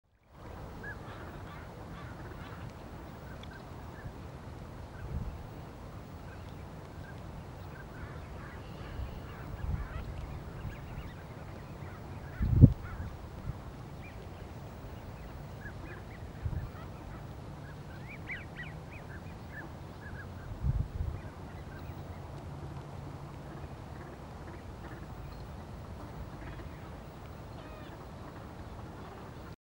Long-Billed Curlew and Marbled Godwit
Blackie Spit, BC

These two rare birds were spotted in the middle of winter a Blackie. They were winter migrants and were stopping by on a particularly cold day. The ducks in the background are the Northern Pintail ducks.